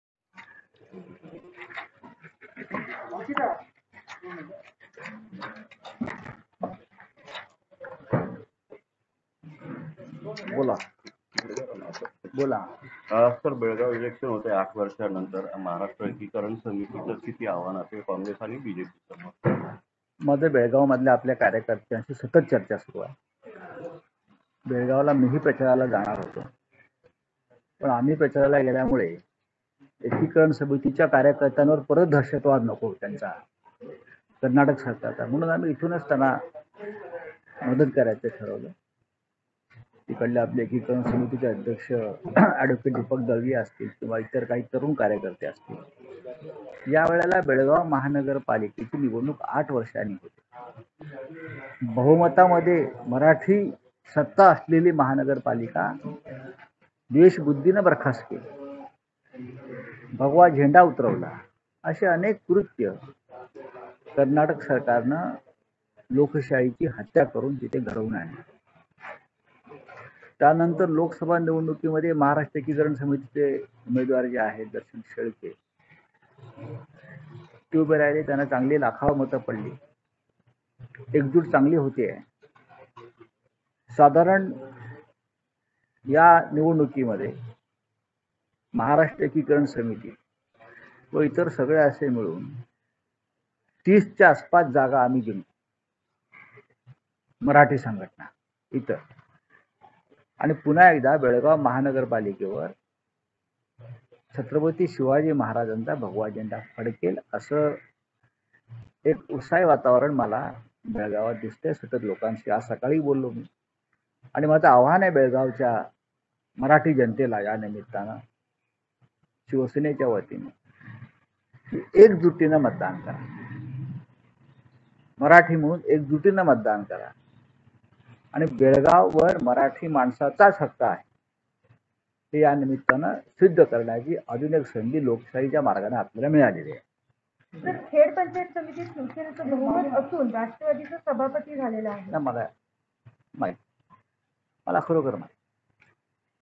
MUMBAI | महाराष्ट्र एकीकरण आणि इतर आमचे मराठी संघटना मिळून 30 च्या आसपास जागा जिंकू. - संजय राऊत
तब्बल 8 वर्षांनी बेळगाव इलेकशन होत आहे

मीही प्रचाराला जाणार होतो

पण आम्ही प्रचाराला गेल्याने एकीकरण समितीवर परत त्यांचा दहशदवाद नको

मराठी सत्ता असलेली महानगरपालिका द्वेषबुद्धीने बरखास्त केली

भगवा उतरवला

असे अनेक कृत्य कर्नाटक सरकारने लोकशाहीची हत्या करून घडवून आणले

लोकसभा निवडणुकीत महाराष्ट्र एकीकरण समितीचे उमेद्वार आहेत त्याना लाखांवर मत मिळाले

या महापालिका निवडणुकीत महाराष्ट्र एकीकरण आणि इतर आमचे मराठी संघटना मिळून 30 च्या आसपास जागा जिंकू

 आणि पुन्हा एकदा बेळगाव महानगरपालिकेवर छत्रपती शिवाजी महाराजांचा भगवा फडकेल

एक जुटीने मतदान करा हे शिवसेनेच्या वतीने मी बेळगावच्या जनतेला आवाहन करतो

मराठी म्हणून एक जुटीने मतदान करा

आणि बेळगाववर मराठी माणसाचा हक्क आहे हे सिद्ध करून दाखवण्यासाठी लोकशाही मार्गाने आणखीन एक संधी या ठिकाणी मिळून आली आहे